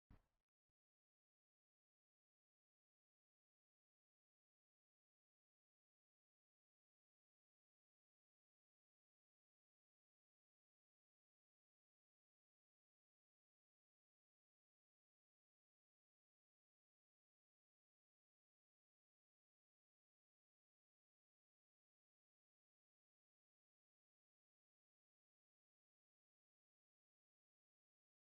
Demolition at Friargate May 2015
The demolition of the old offices on the site of the new Friargate development